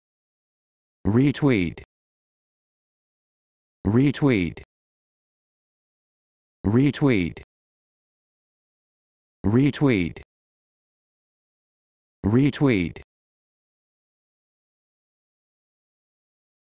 How to Pronounce Retweet
This video shows you how to pronounce Retweet